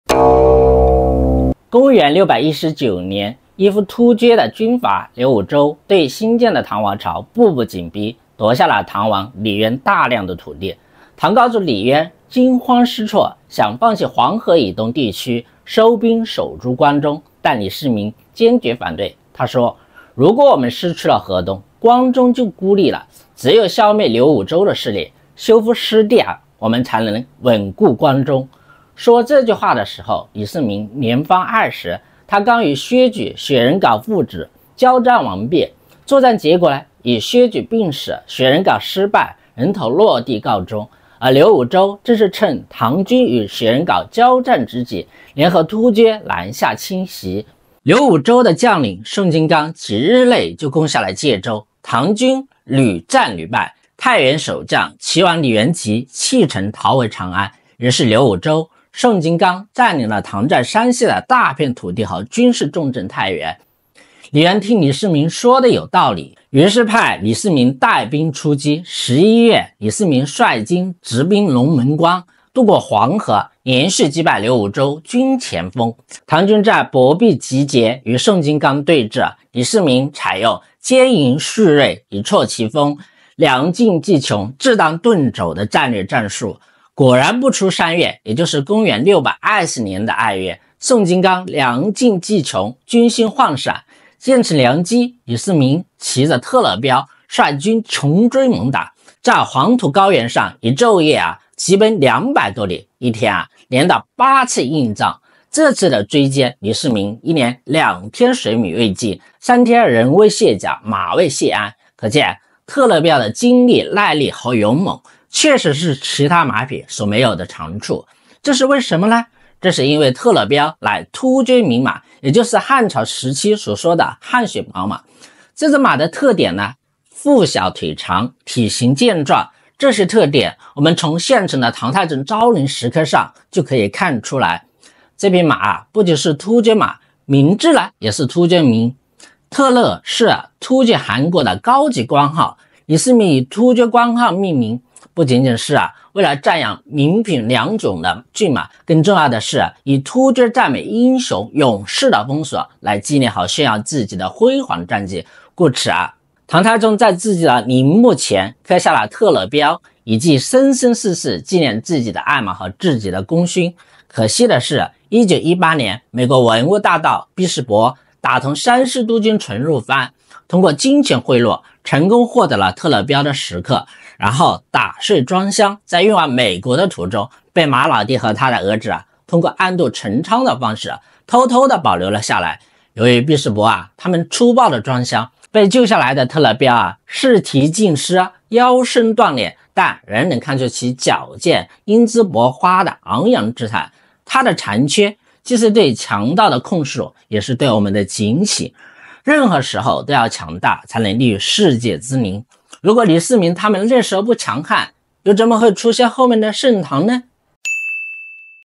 为打败薛举父子，唐太宗骑着特勒骠一夜疾奔200里，谁料1200年后发生了这样的事。历史那些事
公元619年，刘武周乘唐军与薛仁杲交战之际，联合突厥南下侵扰。他的将领宋金刚几日内攻下介州，唐军屡战屡败。太原守将齐王李元吉弃城逃回长安。刘武周、宋金刚遂占领了唐在山西的大片土地和军事重镇太原。唐高祖李渊惊慌，想放弃黄河以东地区，收兵守住关中。但李世民坚决反对，他认为失去河东，关中就孤立了，只有消灭刘武周势力，收复河东失地，才能稳固关中。李渊于是派李世民带兵出击。十一月，李世民率军直奔龙门关，渡过黄河，连续击败刘武周军前锋。唐军在柏壁(今山西新绛县西南)集结，与宋金刚对峙……